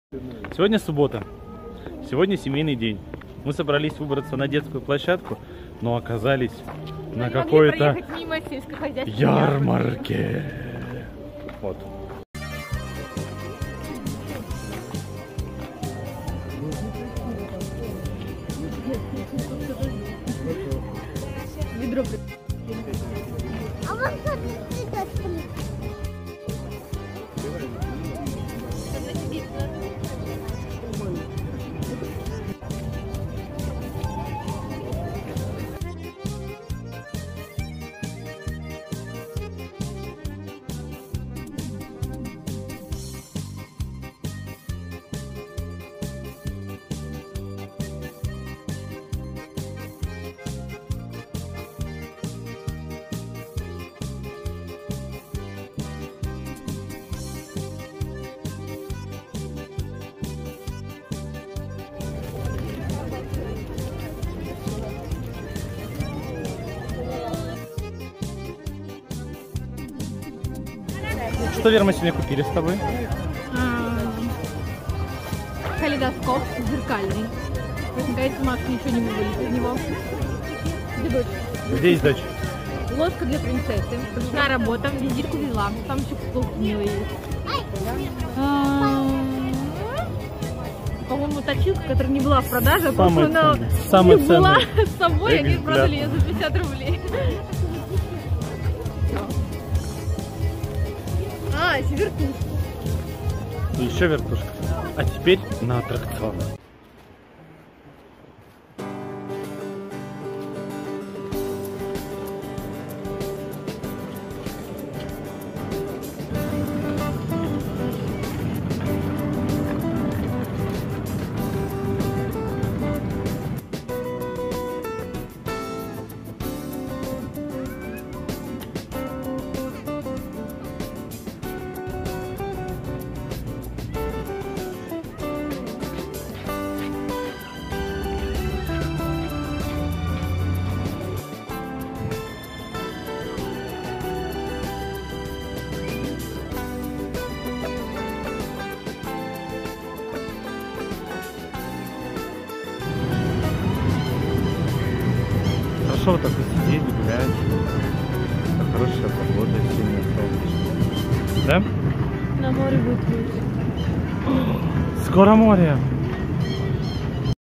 Субботний день. Ярмарка и детские аттракционы.
Небольшая субботняя прогулка с семьёй.

Снято 4K Galaxy S7
Стабилизация DJI osmo mobile